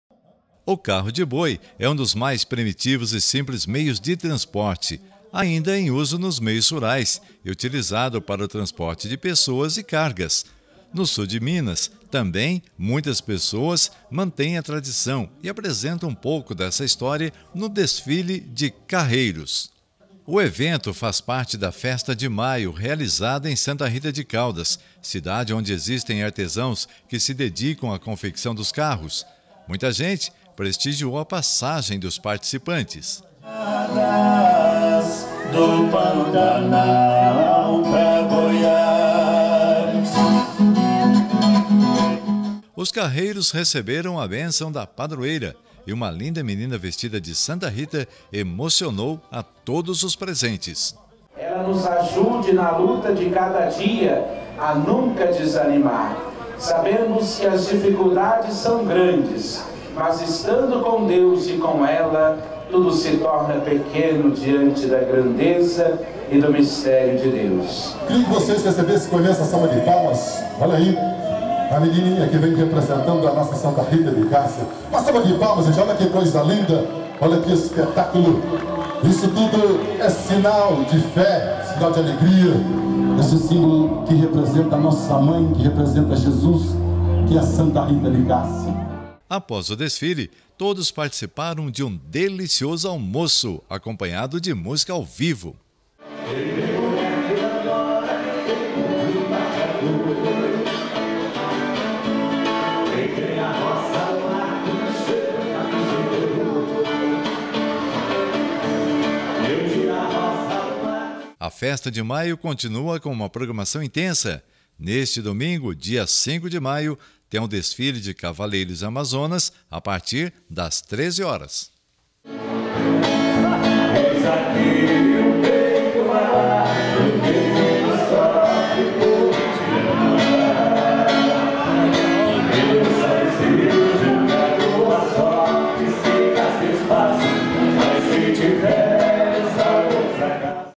Desfile de Carro de Boi movimenta Santa Rita de Caldas
Acontece em Santa Rita de Caldas a tradicional festa de maio, que tem uma programação extensa de eventos. Uma das atrações é bastante curiosa: o desfile de carreiros, que já esta em sua sexagésima terceira edição.